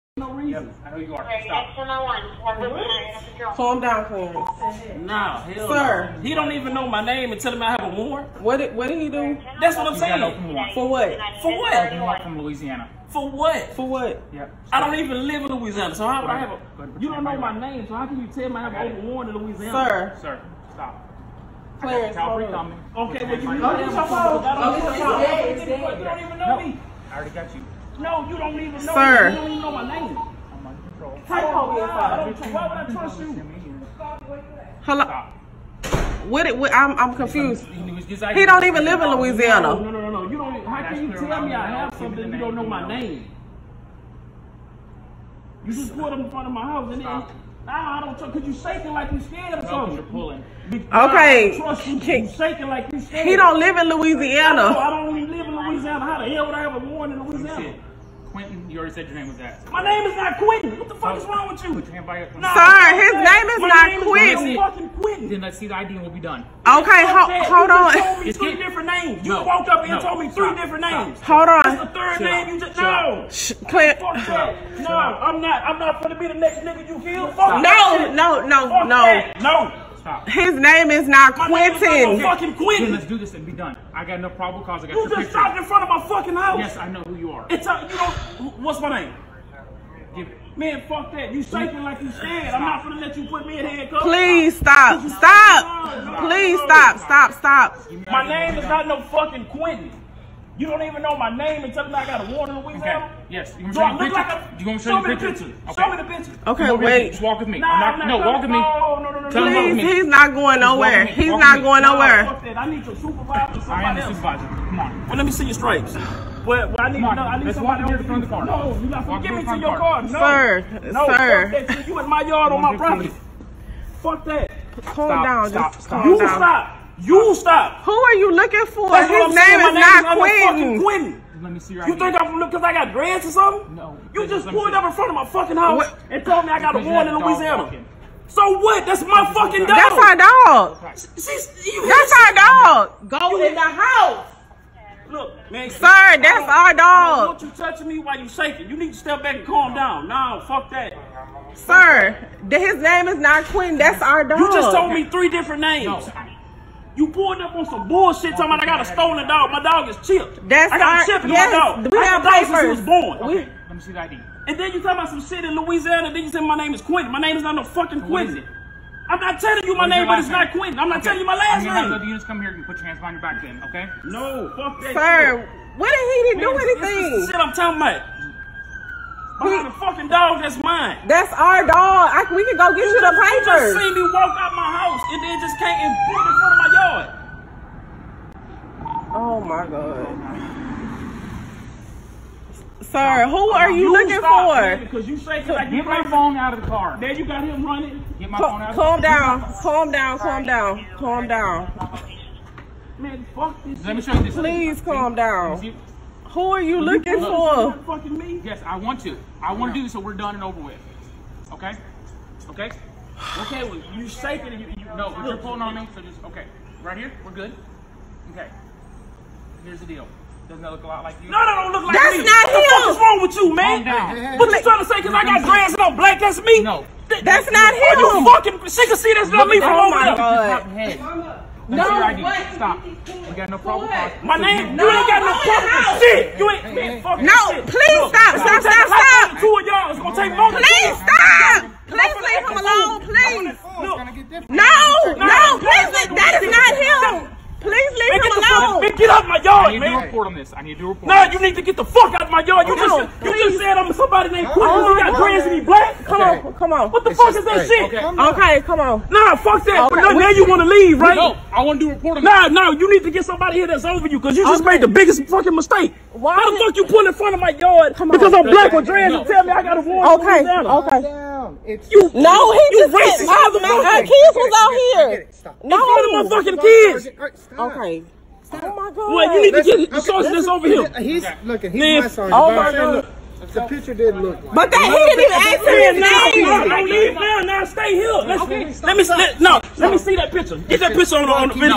Texas Cop Tries to Arrest Wrong Man on Out-of-State Warrant in his Front Yard
A Houston constable spotted a black man with dreadlocks chilling on his front porch and determined he fit the description of another black man with dreadlocks wanted on an out-of-state warrant.